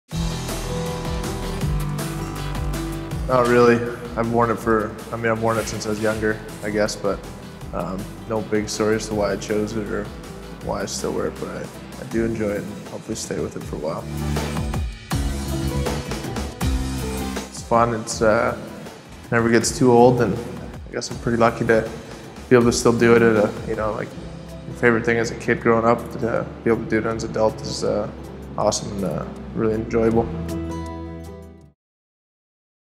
Two Questions with Justin Faulk
Kevin McGran sits down with Carolina Hurricanes Justin Faulk.